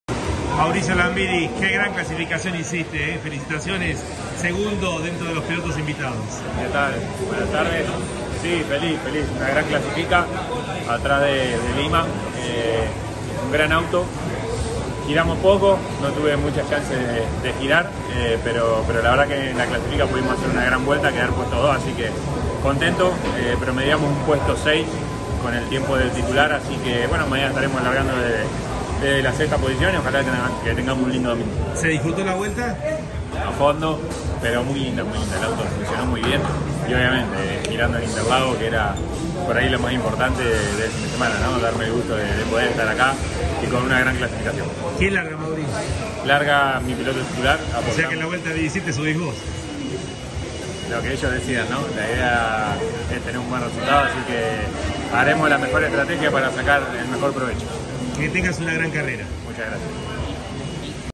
TCR South America: Mauricio Lambiris realizó un gran trabajo en Interlagos
El piloto uruguayo quedò segundo entre los invitados y con ese resultado promedió el sexto lugar de partida en la carrera endurance de San Pablo.